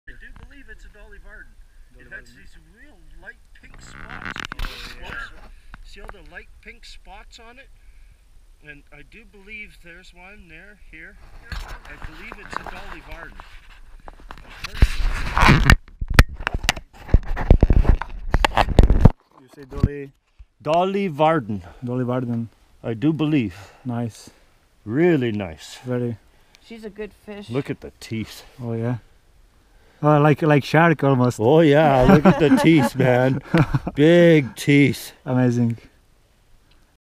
Dolly Varden (Salvelinus malma)
Named after a character in Charles Dickens's novel "Barnaby Rudge" it is found from northern California to Alaska and from Japan to Korea.  The Dolly Varden trout looks somewhat like the Artic Char and often has orange or yellow spots on its sides but the spots are smaller than those on the Artic Char.

The Dolly Varden can attain a weight of over 30 pounds but the usual catch is 2 to 3 pounds.

During summer and fall the wild trout contains more flavor as its fat content is higher than winter and early spring.